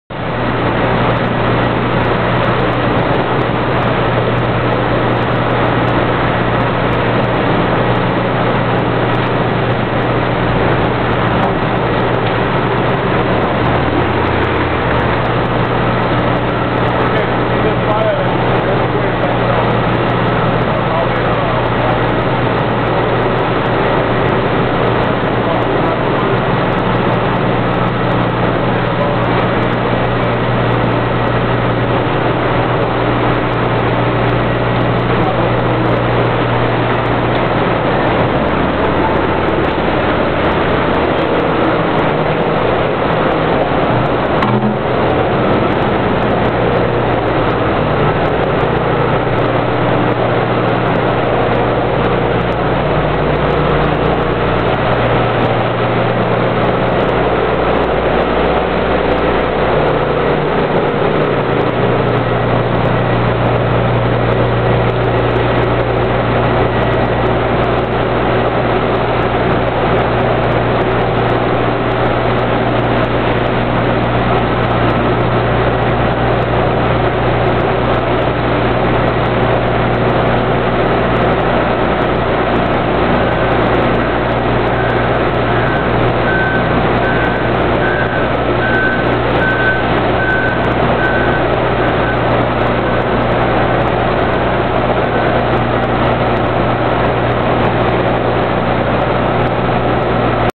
Water Recycling for Aggregate Wastewater
Demonstration of an Alar Water Recycling System for aggregate wastewater.  Dry solids coming off the rotary vacuum drum.

Contact Alar Water Treatment for further info...708/479-6100